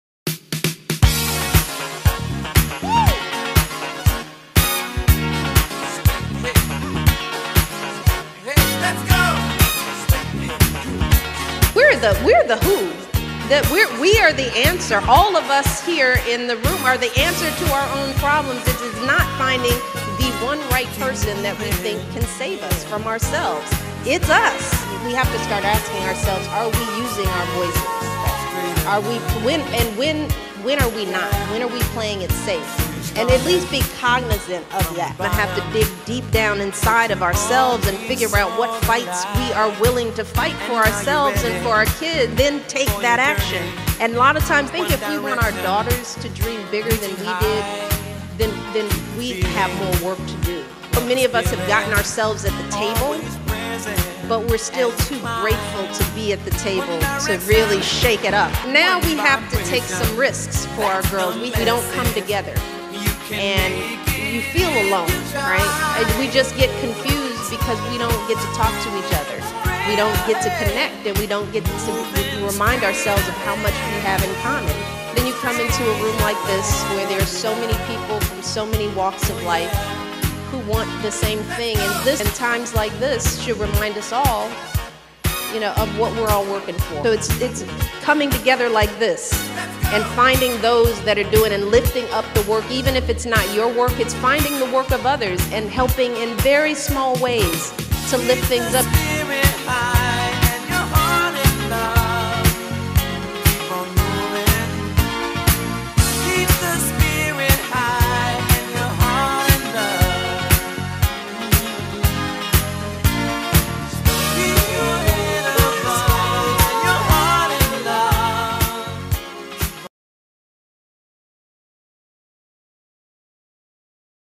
Global Women's Professional and Business Exchange Conference In Accra -Ghana
Join us for this exciting global conference in Ghana on Saturday October 6th 2018 at Kempinski Hotel In Accra. or email .